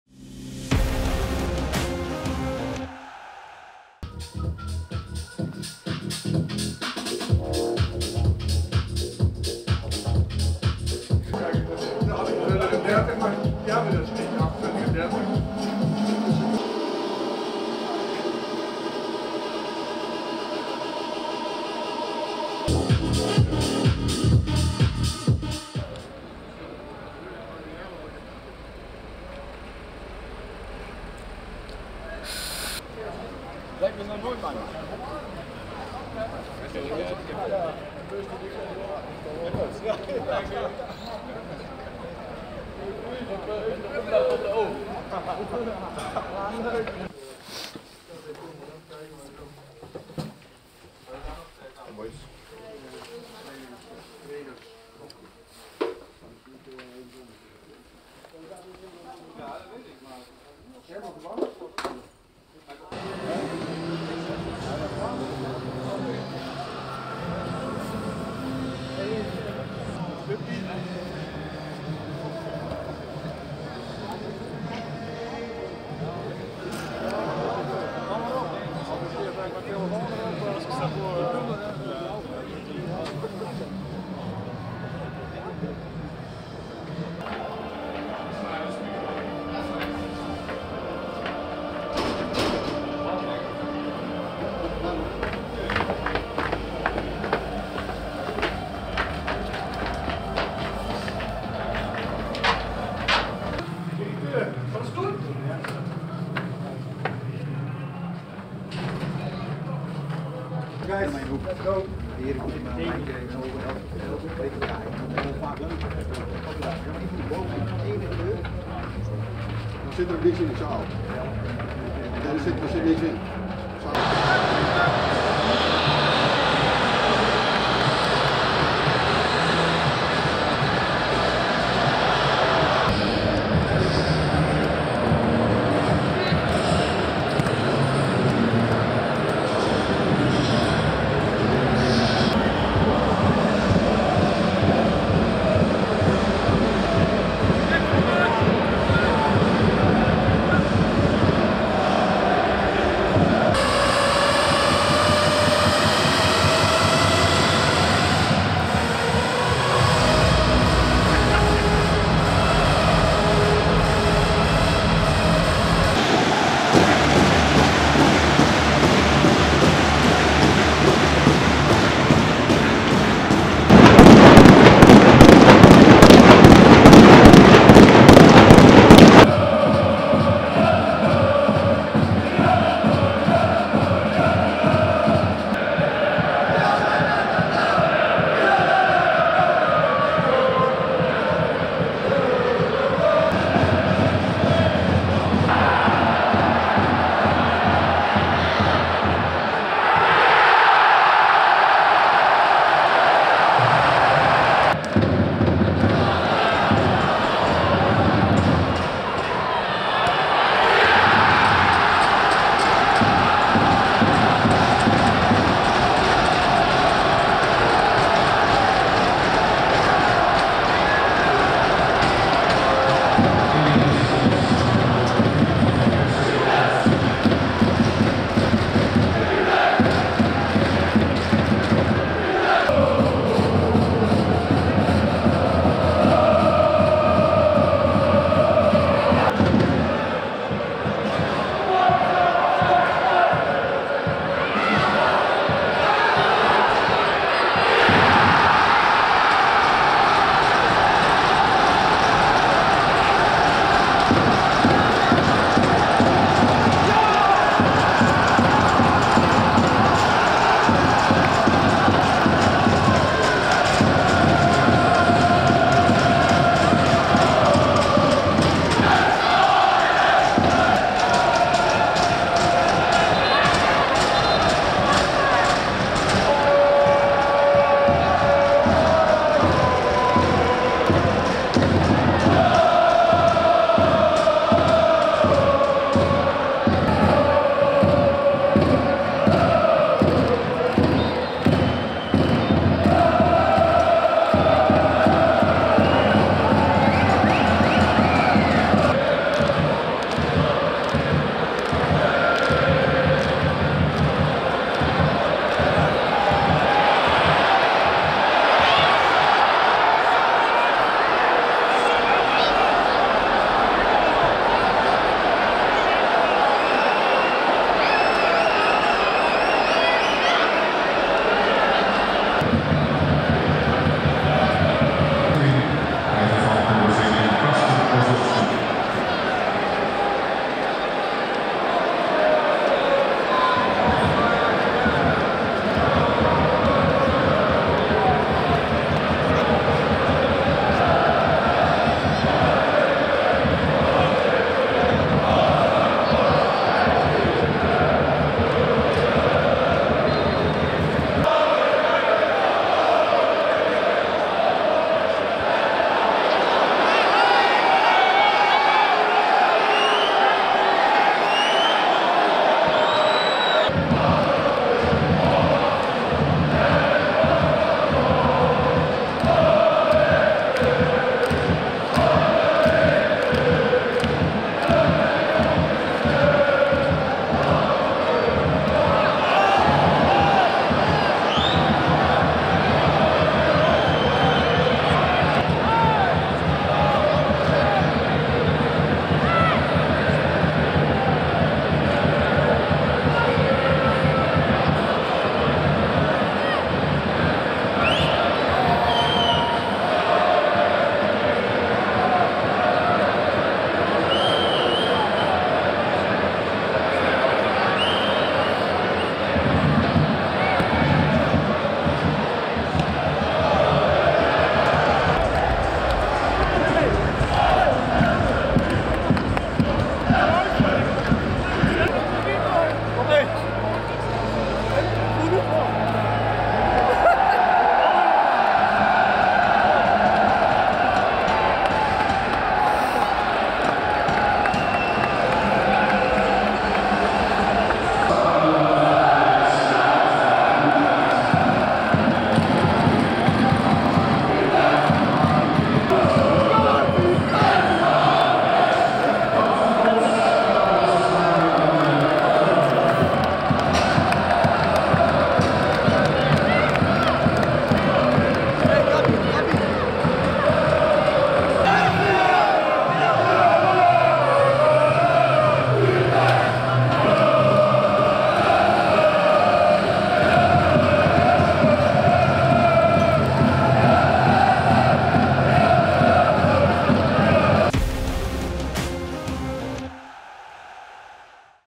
CLUBCAM | On our way up! ⬆️ Pröpper is back! 🔙 Win in Heerenveen! 😍
Vitesse heeft een mooie avond beleefd in Friesland. De Arnhemmers wonnen op bezoek bij sc Heerenveen met 1-3. Bartosz Bialek, Gabriel Vidović en Million Manhoef waren verantwoordelijk voor de doelpuntenproductie. De Arnhemse formatie speelde een goede wedstrijd en werd dus ook eindelijk beloond met drie punten. Davy Pröpper maakte in de 66ste minuut zijn rentree. Bekijk hier de clubcam beelden.